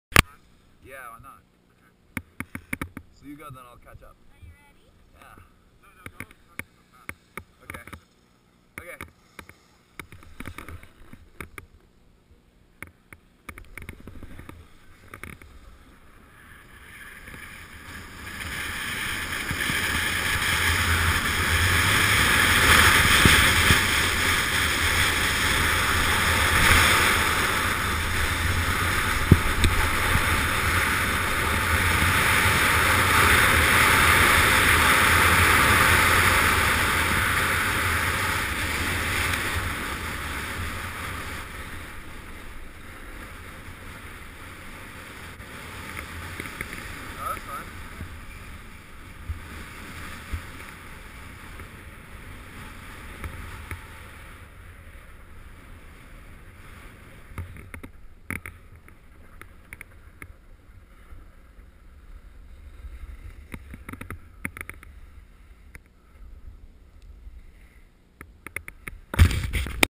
Speed Run Sierra at Tahoe
Going 50.0 mph down Castle at the beautiful Sierra at Tahoe on a bluebird day!